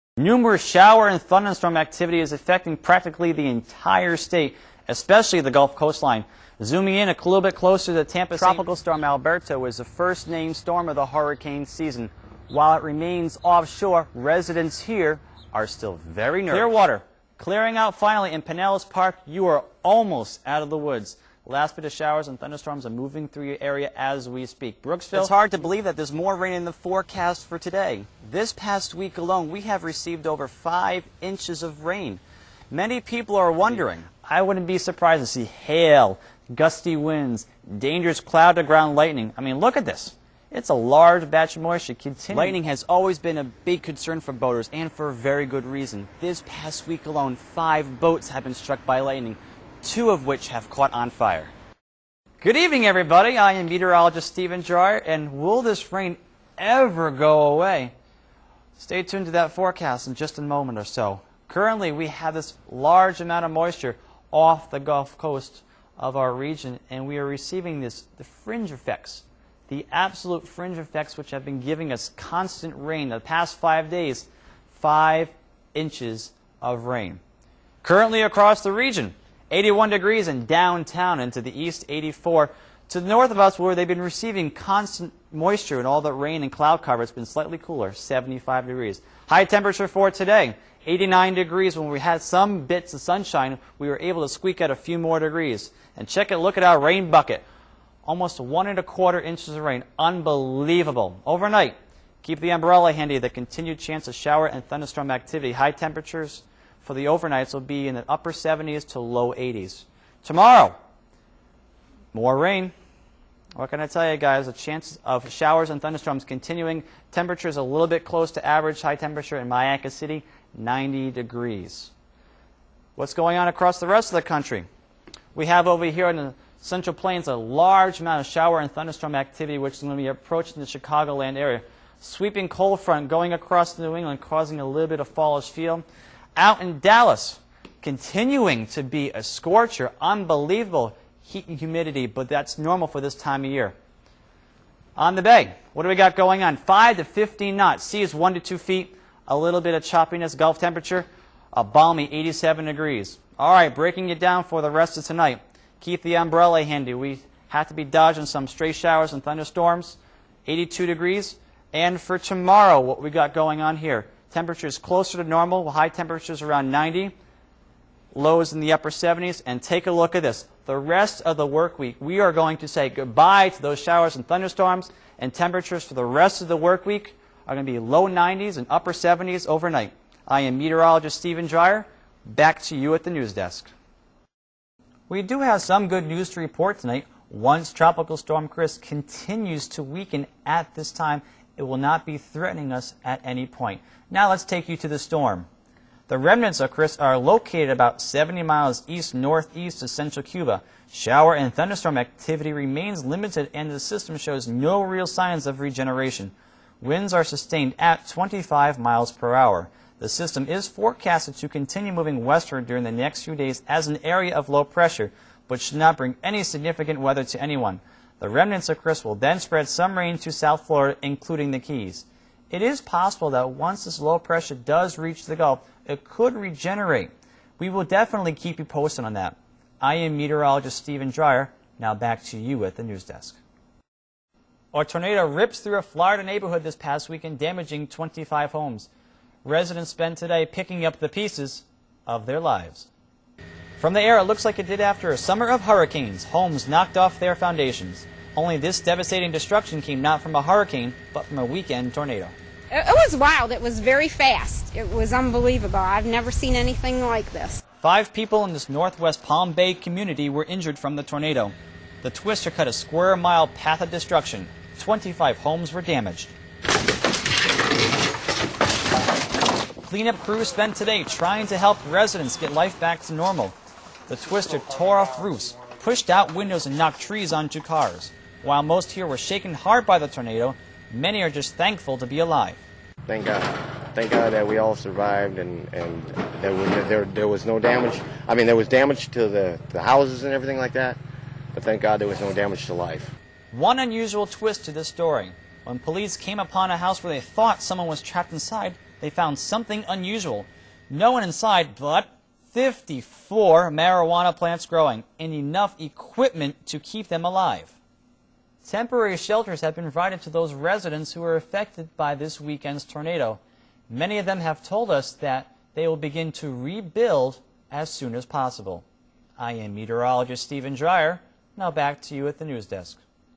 Stephen Dryer Meteorologist Resume Video
Stephen Dryer compilation of weather bits including reporting, green screen, and in the weather center